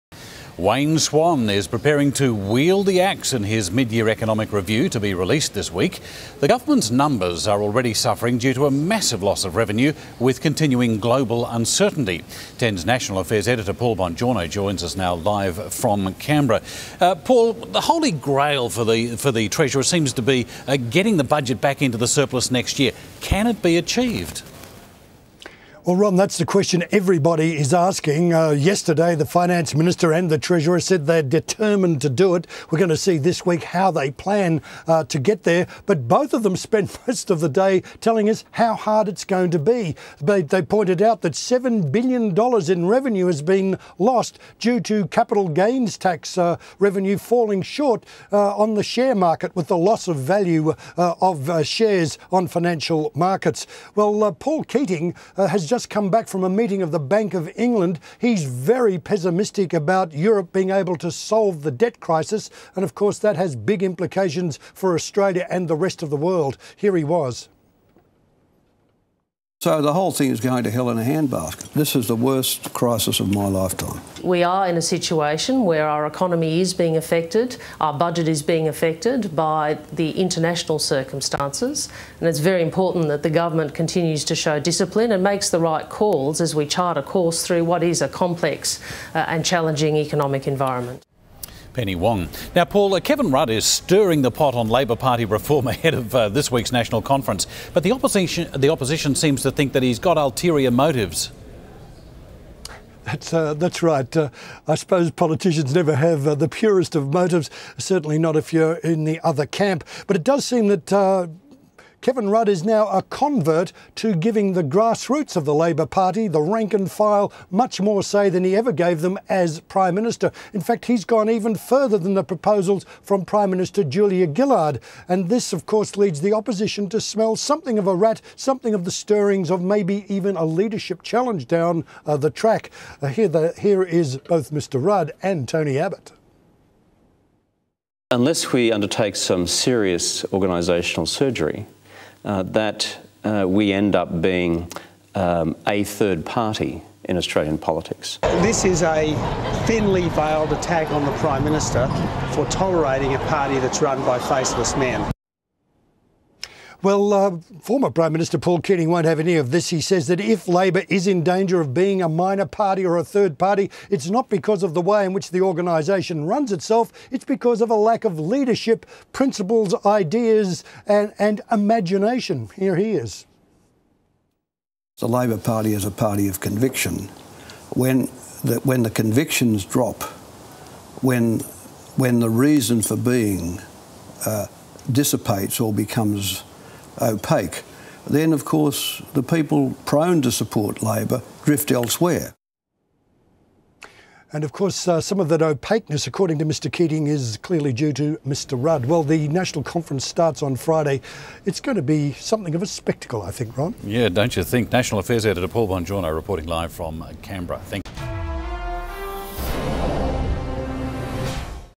Swan's midyear review
Wayne Swan is preparing to wield the axe in his mid year economic review to be released this week.